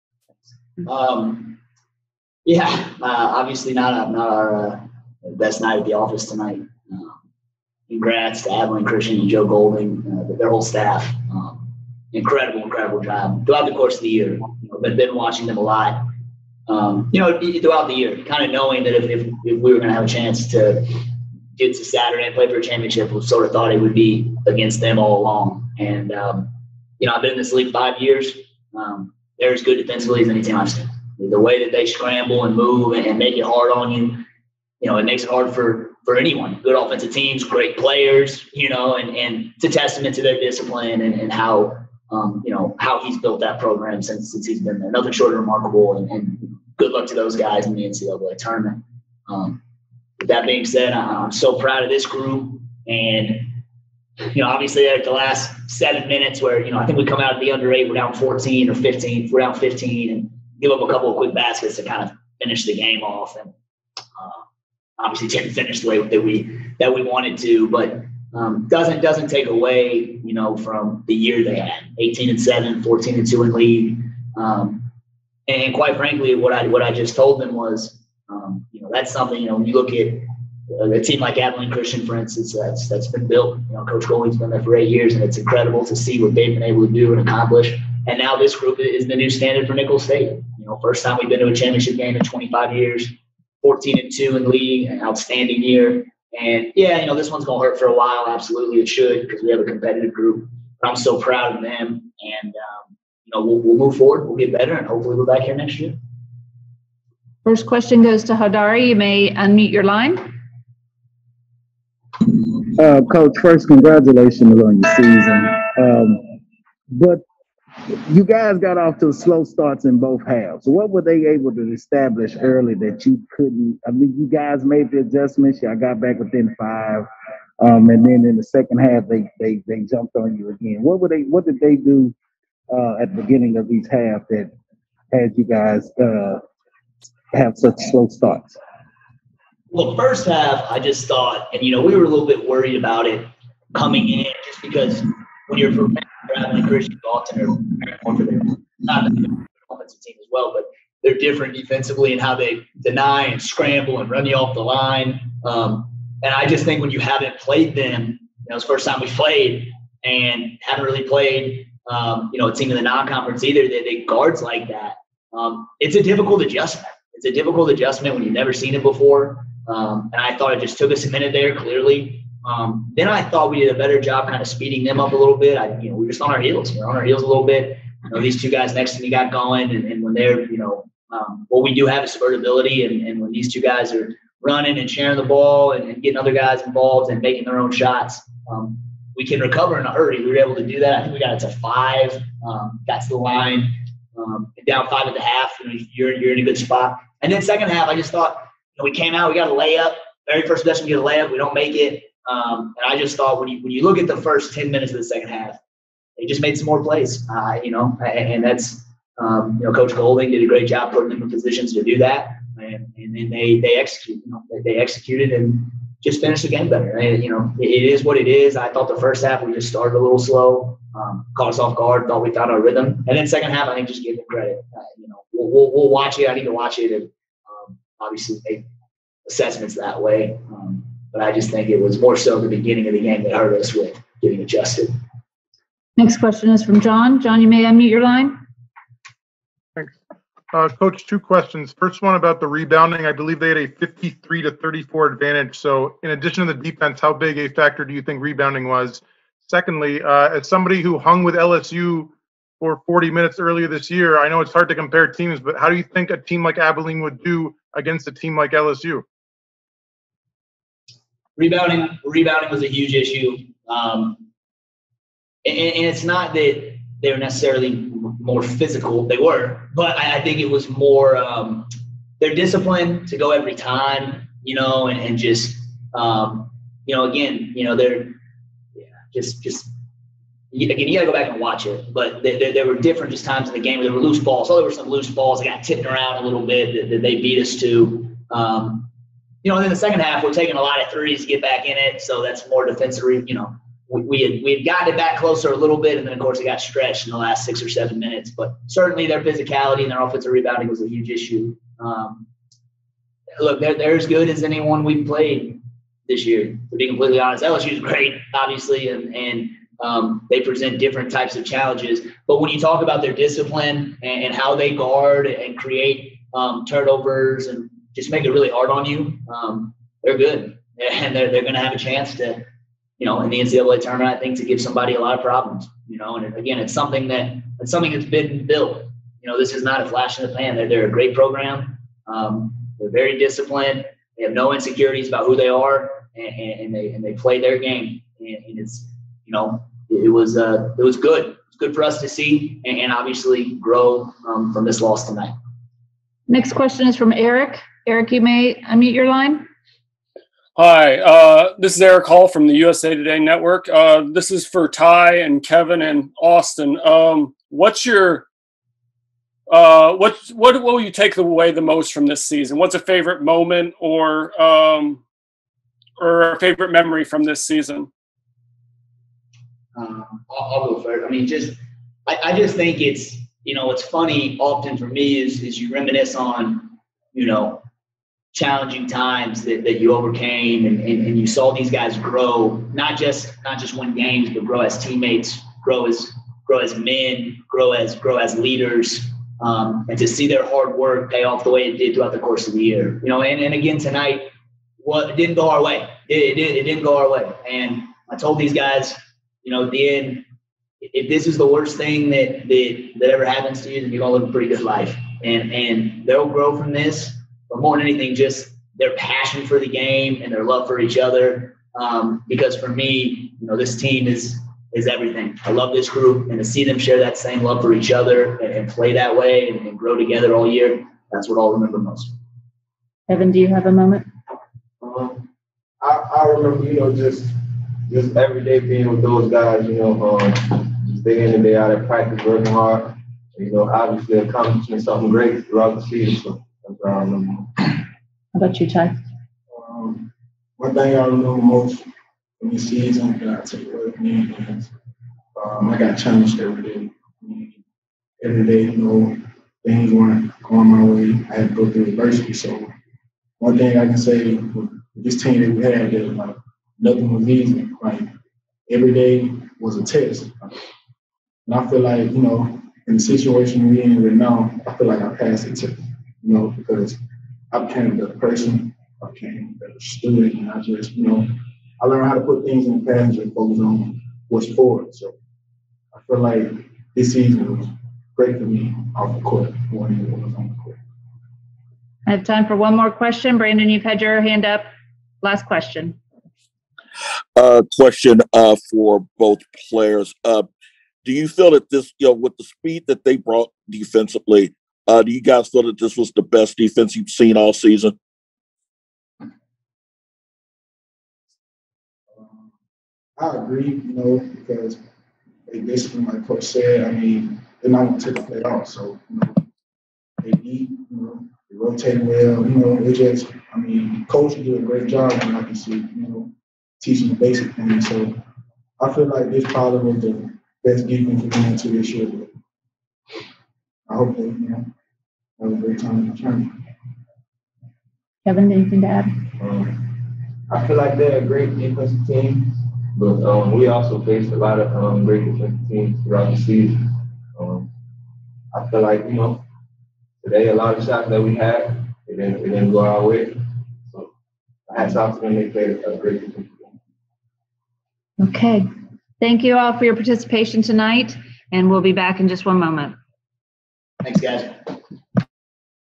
Southland MBB Tournament Championship Game Nicholls Postgame Press Conference | The Garage Apartment
Southland Conference regular season champs Nicholls Colonels discuss their tough loss in the conference tournament championship game.